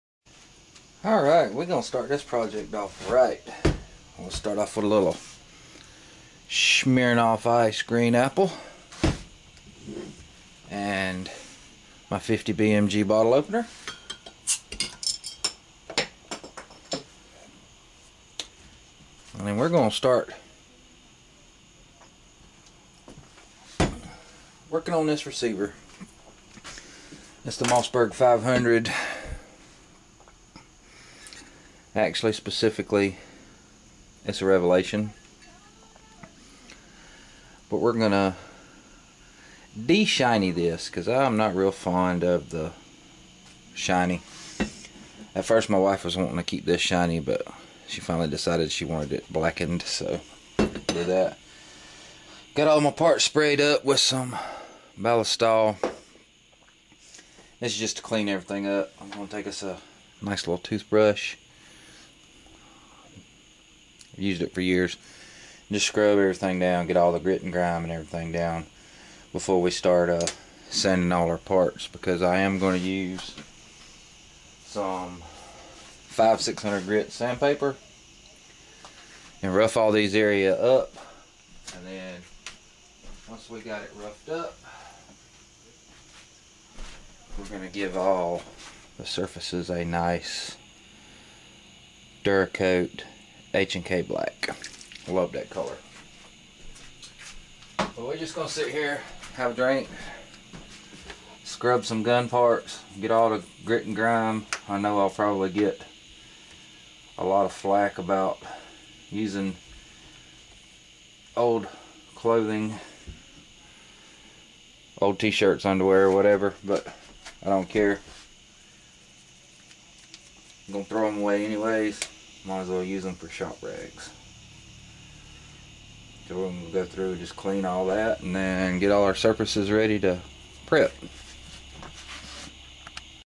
Mossberg 500 pt1
Got her all stripped down and ready to start prepping the  metal for refinishing.